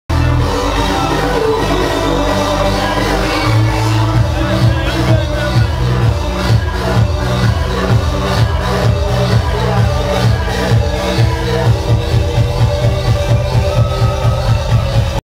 Warehouse - Sing your favourite song?
O2 asked fans at O2 Academy Oxford: "Sing your favourite song?"